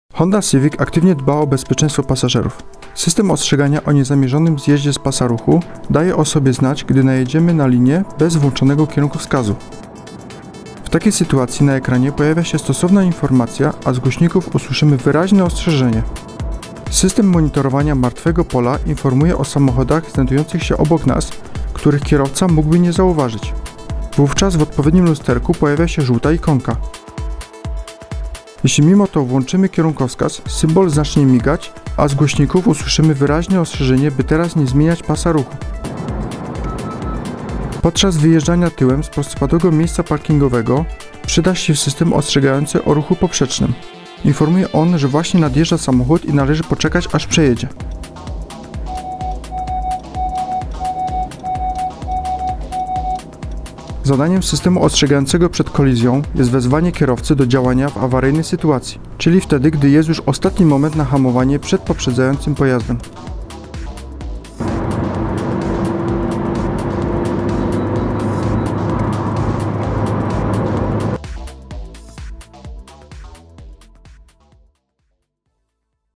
Honda Civic: aktywne systemy bezpieczeństwa :: [1001cars]
Honda Civic aktywnie dba o bezpieczeństwo pasażerów. System ostrzegania o niezamierzonym zjeździe z pasa ruchu daje o sobie znać, gdy najedziemy na linię bez włączonego kierunkowskazu. W takiej sytuacji na ekranie pojawia się stosowna informacja, a z głośników usłyszymy wyraźne ostrzeżenie.

System monitorowania martwego pola informuje o samochodach znajdujących się obok nas, których kierowca mógłby nie zauważyć. Wówczas w odpowiednim lusterku pojawia się żółta ikonka.

Jeśli mimo to włączymy kierunkowskaz, symbol zacznie migać, a z głośników usłyszymy wyraźne ostrzeżenie by teraz nie zmieniać pasa ruchu.

Podczas wyjeżdżania tyłem z prostopadłego miejsca parkingowego przyda się system ostrzegający o ruchu poprzecznym. Informuje on, że właśnie nadjeżdża samochód i należy poczekać aż przejedzie. Warto zaznaczyć, że ostrzeżenia przekazywane są odpowiednio wcześnie.

Zadaniem systemu ostrzegającego przed kolizją jest wezwanie kierowcy do działania w awaryjnej sytuacji. Czyli wtedy gdy jest już ostatni moment na hamowanie przed poprzedzającym pojazdem.